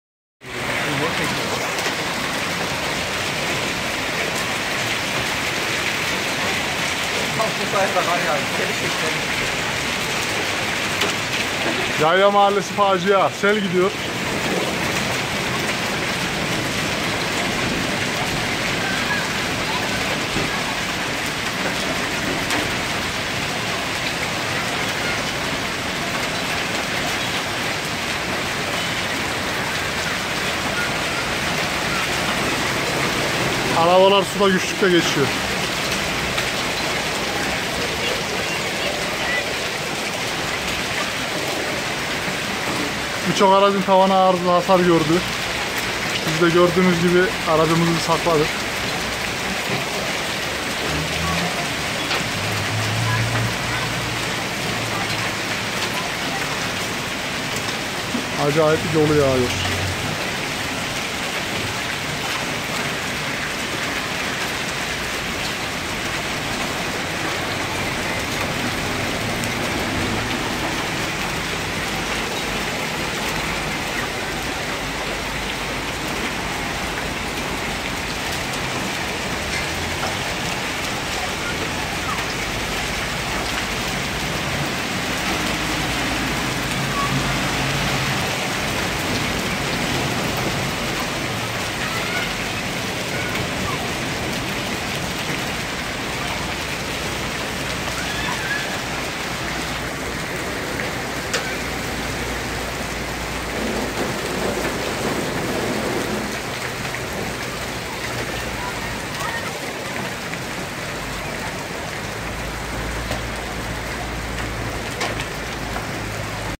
Arabanızı toludan nasıl korursunuz!
Ankara kecogren yayla mah. Yoğun yolu yağışı sonucu sel